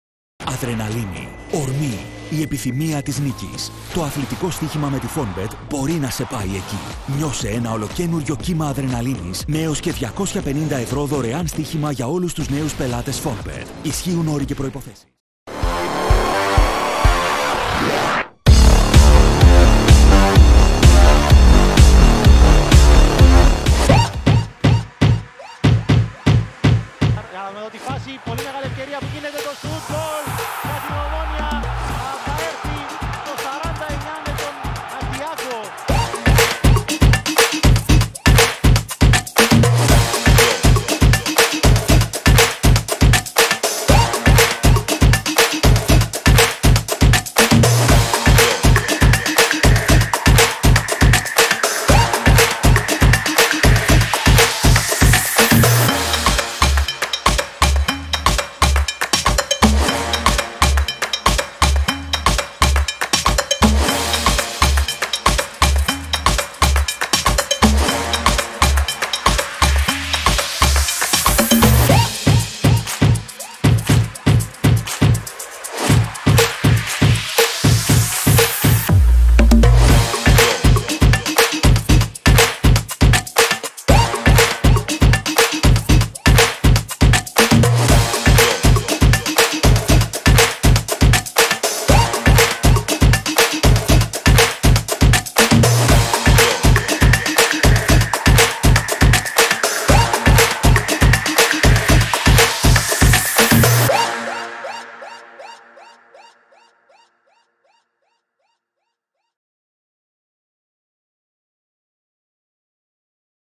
Από πλάγιο άουτ σε πέντε δευτερόλεπτα γκολ από τον Τιάγκο!
Μακρινή εκτέλεση πλαγίου άουτ από τον Λέτσιακς, το πλασέ του Εζρά αποκρούεται και ο Τιάγκο στην επαναφορά σκοράρει το 1-0 για την ΟΜΟΝΟΙΑ στο παιχνίδι με τον Απόλλωνα για την 4η αγωνιστική του πρωταθλήματος 2019-20!